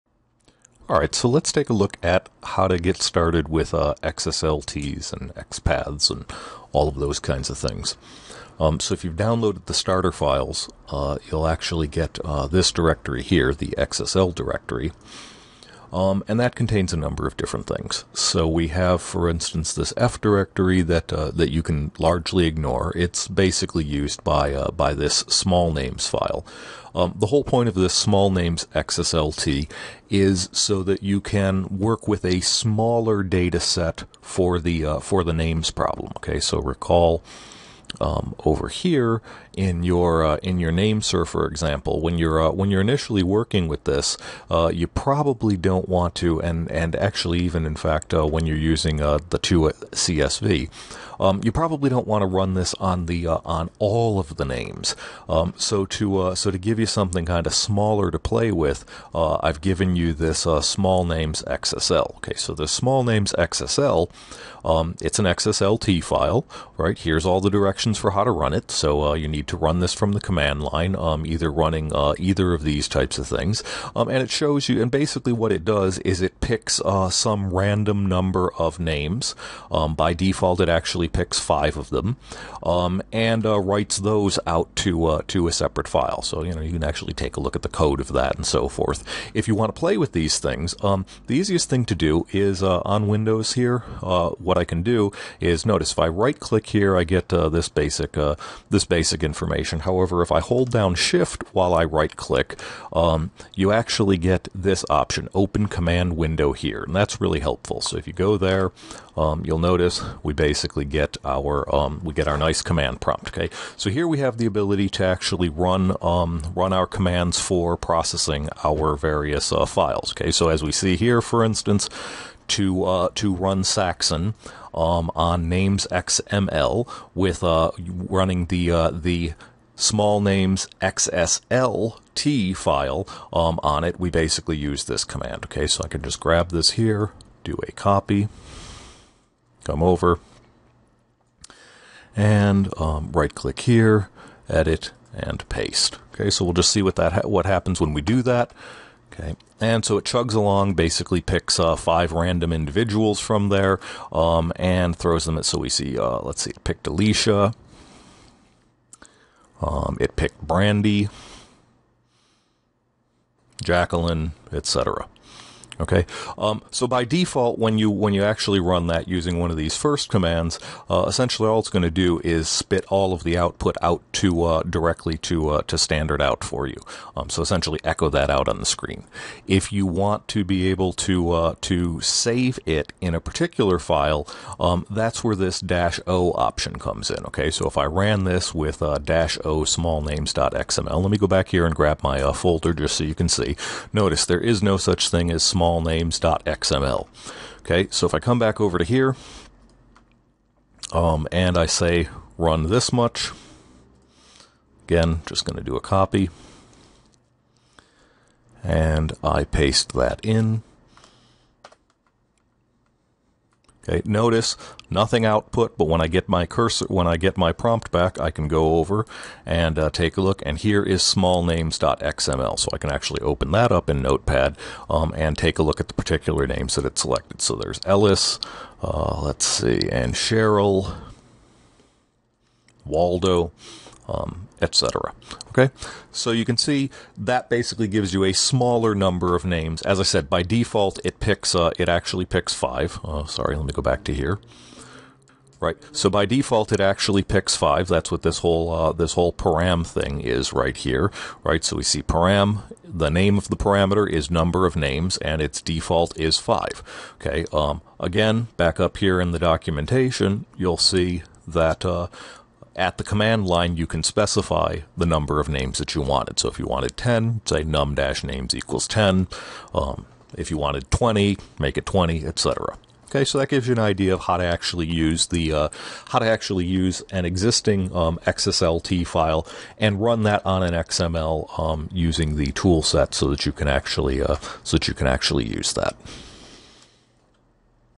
Getting Started with XSLT and Saxon
How to use the Saxon XSLT and XQuery processor